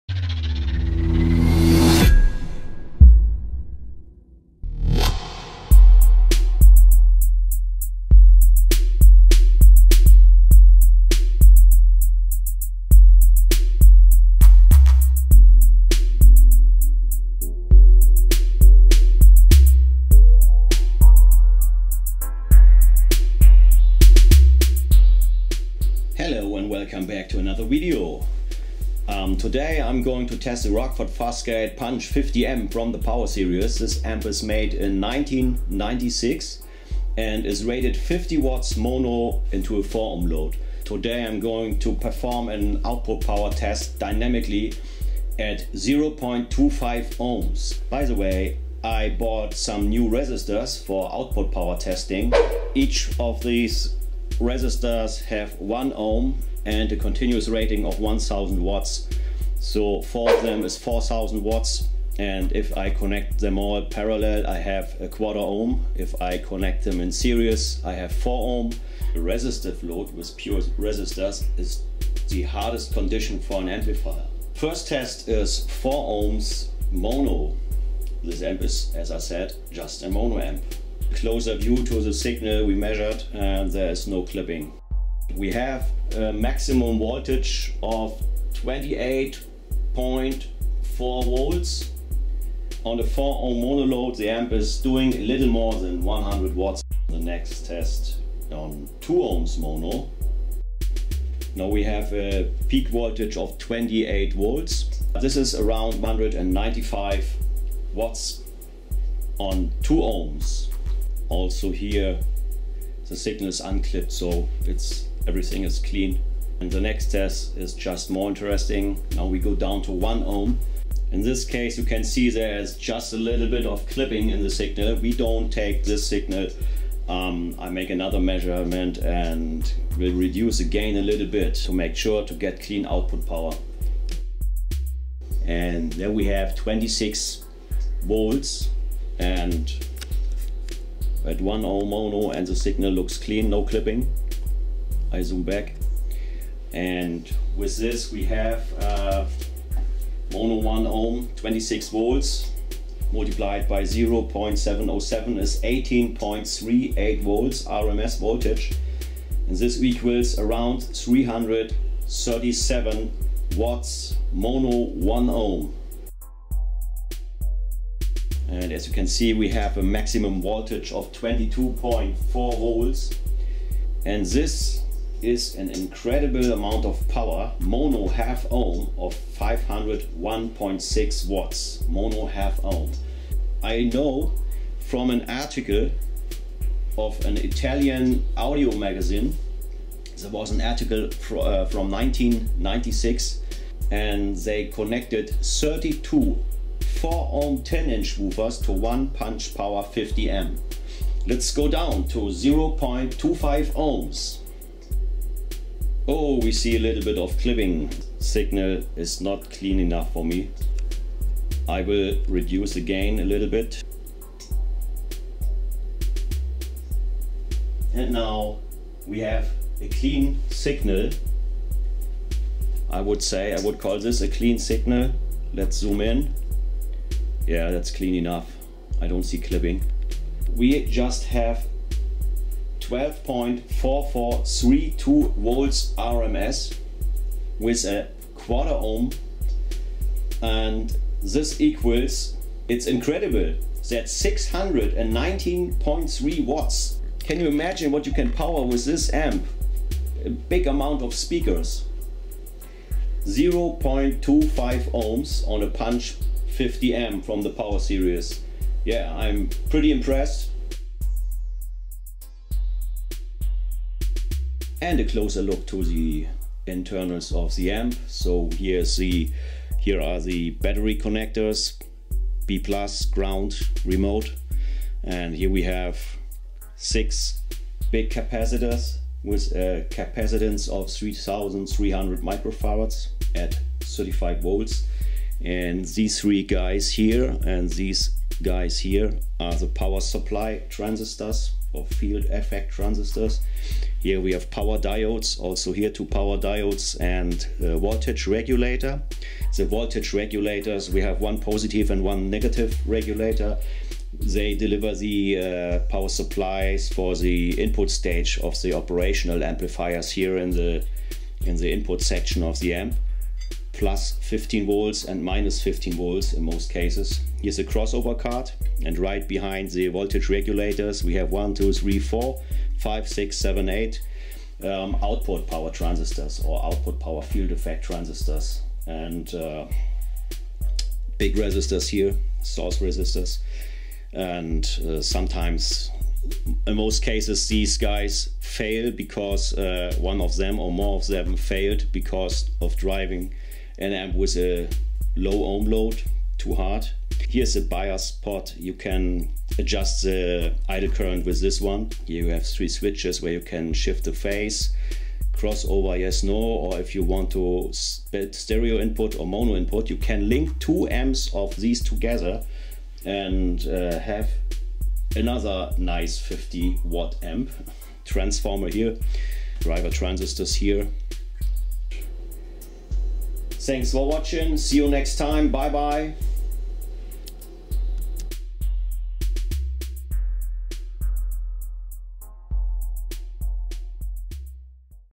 Rockford Fosgate Punch Power 50M amp dyno run old school cheater amplifier 0.25 low ohm test & guts
In this video i show an old school cheater mono amp from 1996 made by rockford fosgate.
Rockford rated this amp 200 watts @ 1 ohm.
I found an old article from an italian audio magazine where they were at the RTTI and tested the Power 50m with 32 10" 4 ohm woofers.
At the End of this clip i show some internal components of this amp

Note that this is a resistive load test which is harder for the amp than with speakers which impedance is higher and not fixed.
Tested with 1 kHz burst signal.
I introduce my new dummy load, now i got 8 resistors, 4 x 1 ohm resistors 1000 watts each and another 4 resistors with 3.9 ohms 1000 watts each.
With these resistors i can perform tests with up to 8000 watts of continous power or 24.000 watts short term power.

Pics shot with the leica nocticon lens